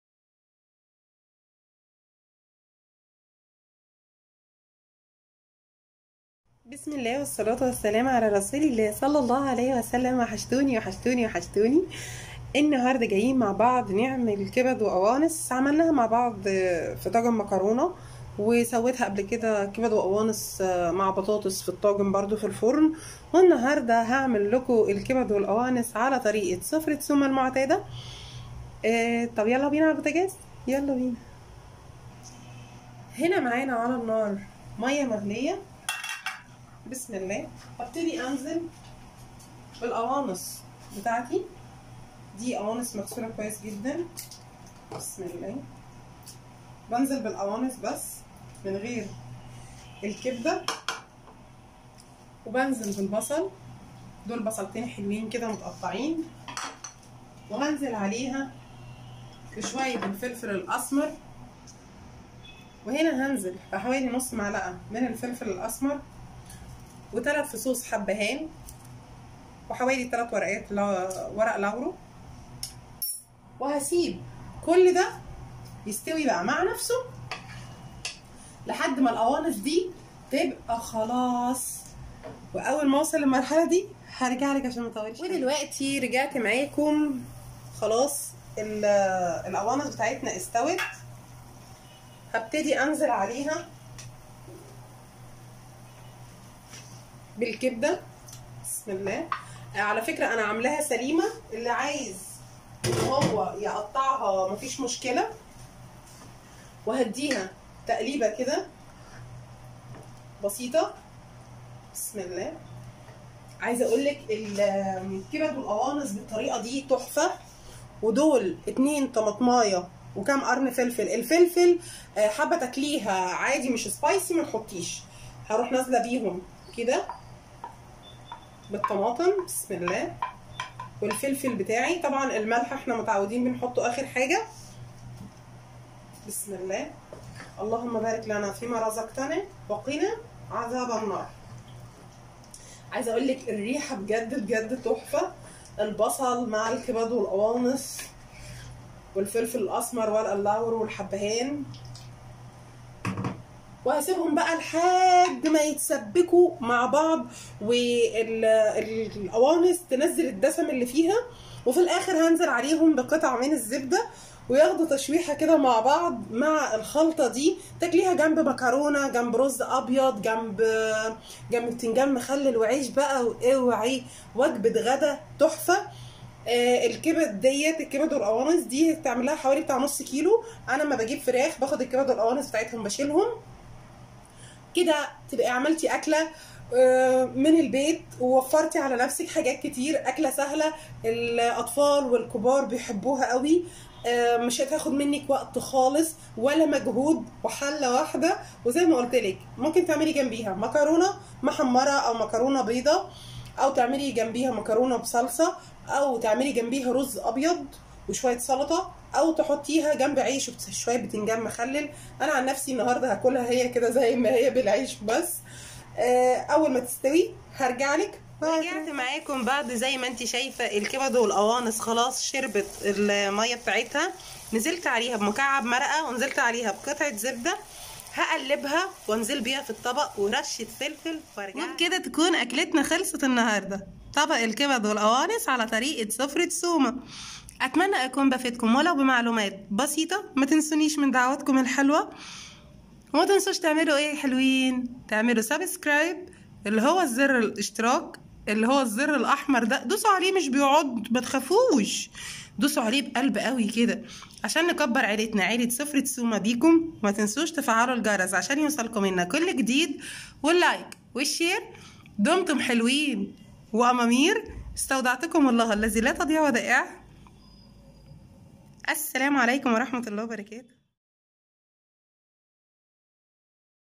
طريقه الكبد والقوانص المعتاده بطعم جنان (سفره سومه)
احلى وأسهل طريقه لعمل الكبد والقوانص
مفيش أسهل من كده